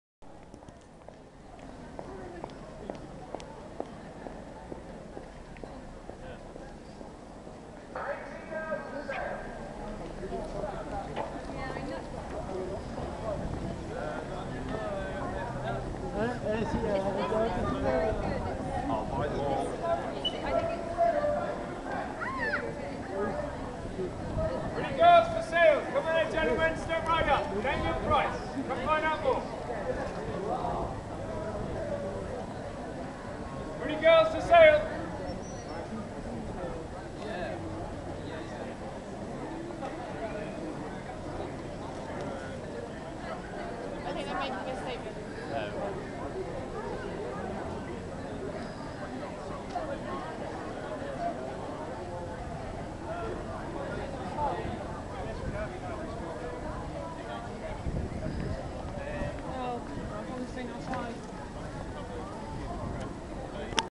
PRETTY WOMAN FOR SALE
Esta manifestación de protesta la encontré en the cornmarket street de Oxford